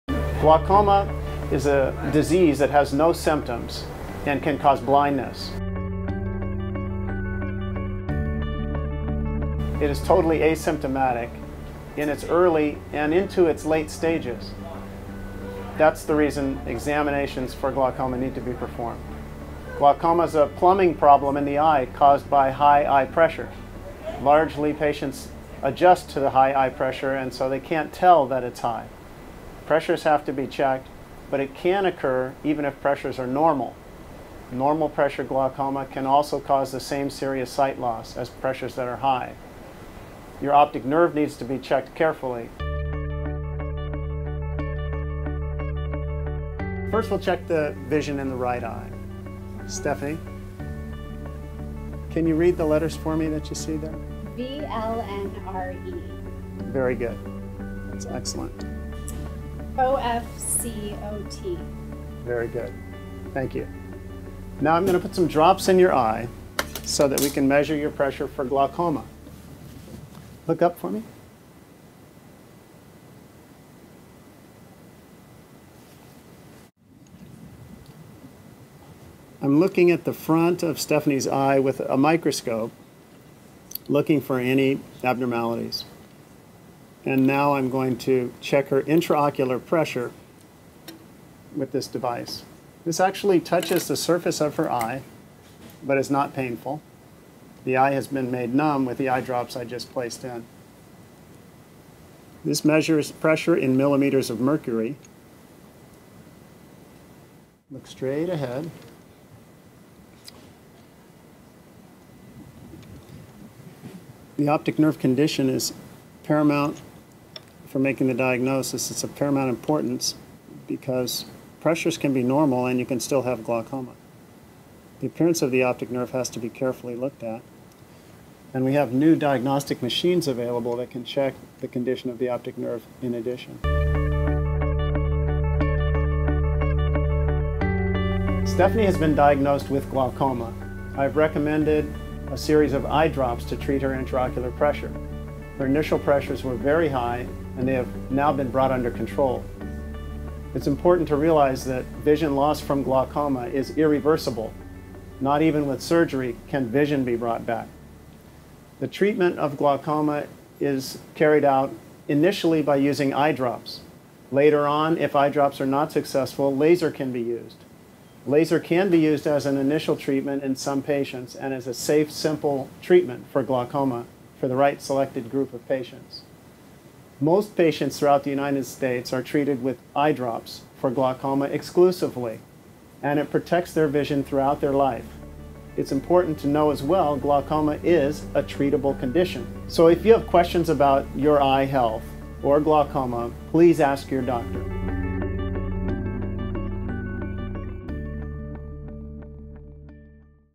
Learning About Glaucoma
Dr. A. Sydney Williams discusses glaucoma and walks us through a typical eye examimation.